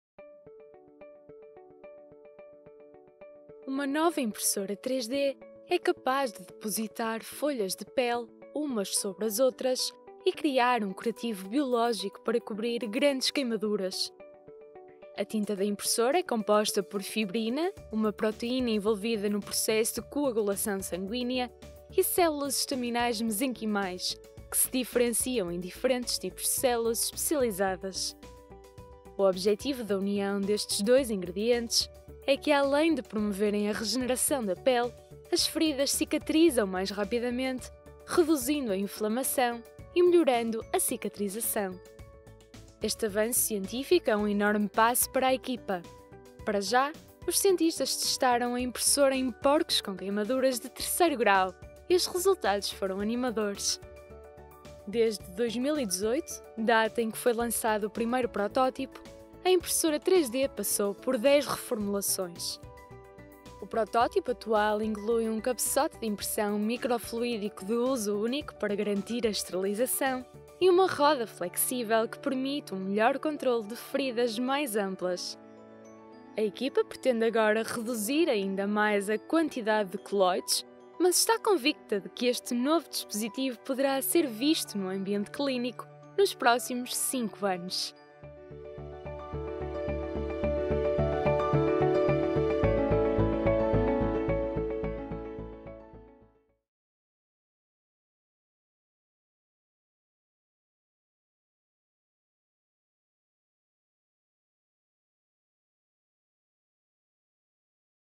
“Impressora de pele” promete ajudar na cicatrização de queimaduras
Uma nova impressora 3D é capaz de depositar "folhas de pele", umas sobre as outras  , e criar um curativo biológico para cobrir grandes queimaduras.
A tinta da impressora é composta por fibrina (uma proteína envolvida no processo de coagulação sanguínea) e células estaminais mesenquimais que se diferenciam em diferentes tipos de células especializadas.
O objetivo da união destes os dois "ingredientes" é que, além de promoverem a regeneração da pele, as feridas cicatrizam mais rapidamente, reduzindo a inflamação e melhorando a cicatrização.
Este avanço científico é um enorme passo para a equipa. Para já, os cientistas testaram a impressora em porcos com queimaduras de terceiro grau, e os resultados foram animadores.
Desde 2018, data em que foi lançado o primeiro protótipo, a impressora 3D passou por 10 reformulações.
O protótipo atual inclui um cabeçote de impressão microfluídico de uso único para garantir a esterilização, e uma roda flexível, que permite um melhor controlo de feridas mais amplas.

A equipa pretende reduzir ainda mais a quantidade de quelóides, mas está convicta de que este novo dispositivo poderá ser visto num ambiente clínico nos próximos cinco anos.

FOOTAGE Universidade de Toronto | Envato Elements | Videvo